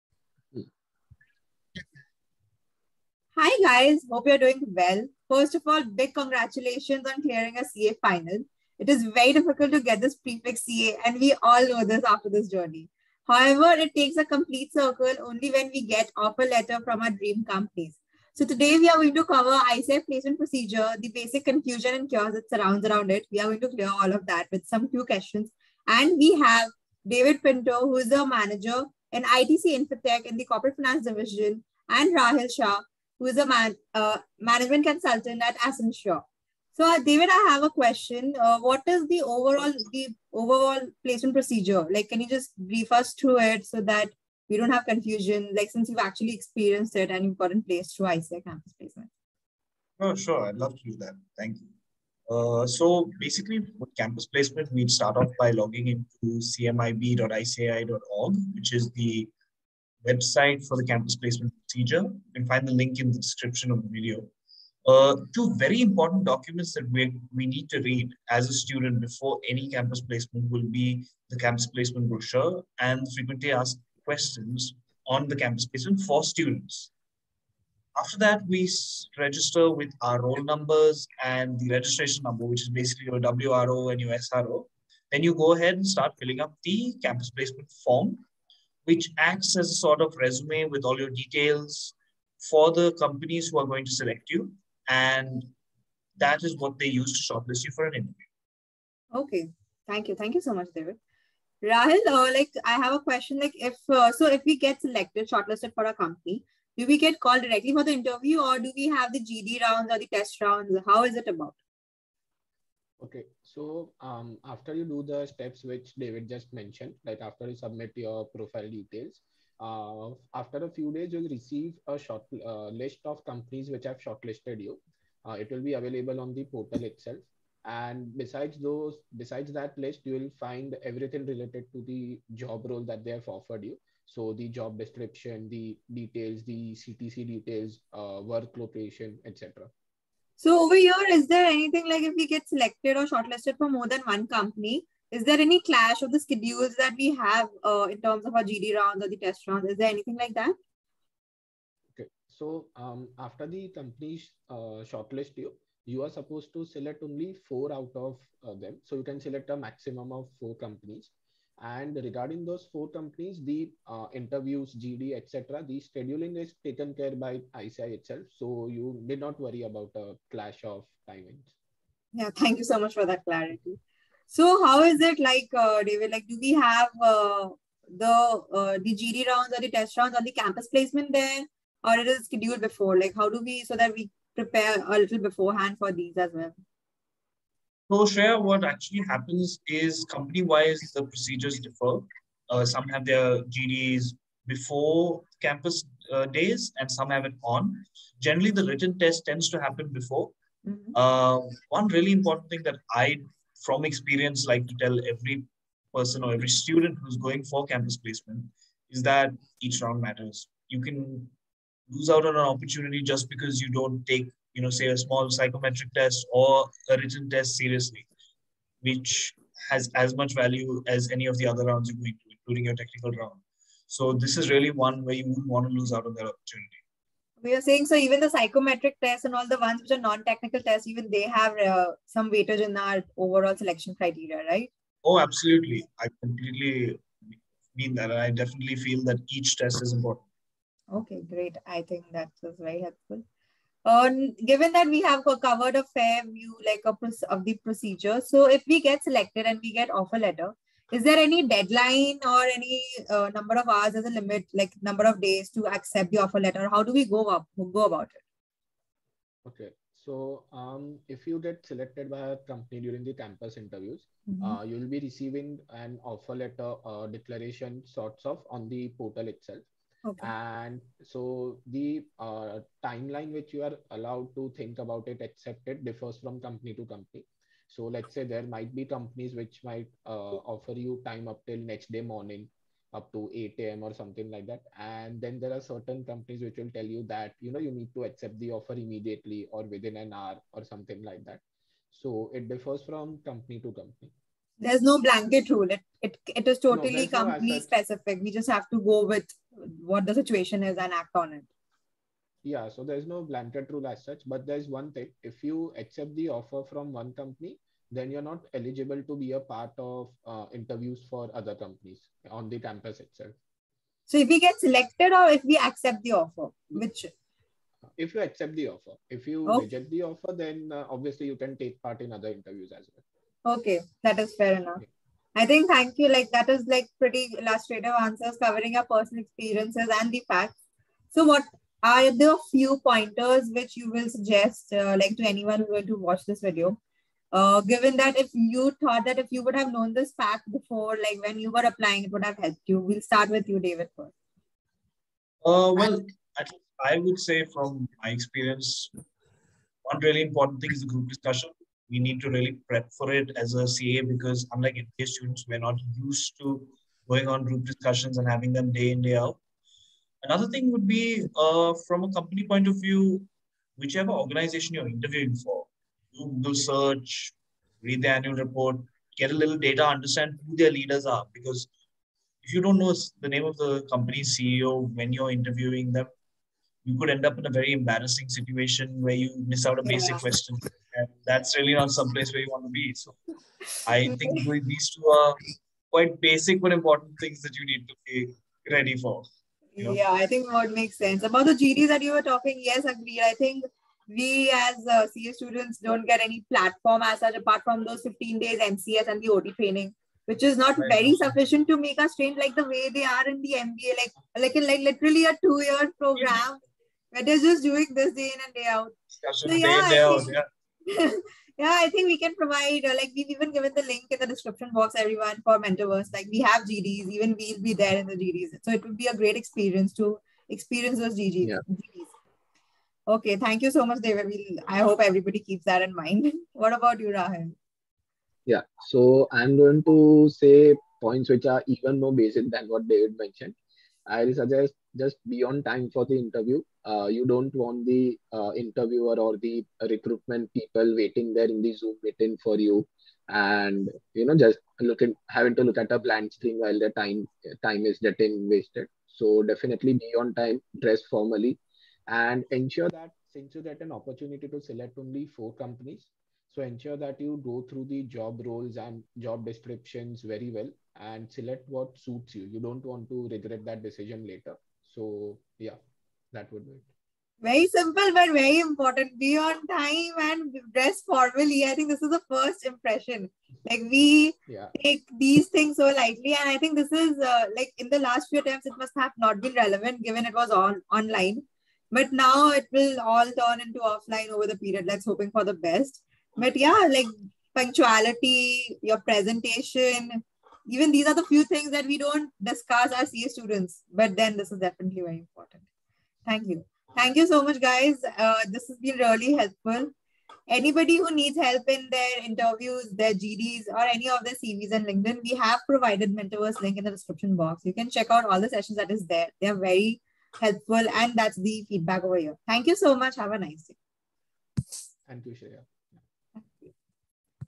ICAI Campus Placement explained in detail
Questions covered -

1.Brief on Campus placement procedure.
       A. Important documents to be read -
          a. Campus Placement Brochure
          b. FAQs
2.Procedure followed after getting shortlisted
3.Schedule for GD/Test rounds
4.Procedure to be followed after getting offer letter
5.Last few pointers to be kept in mind!

All the best for your placement procedure!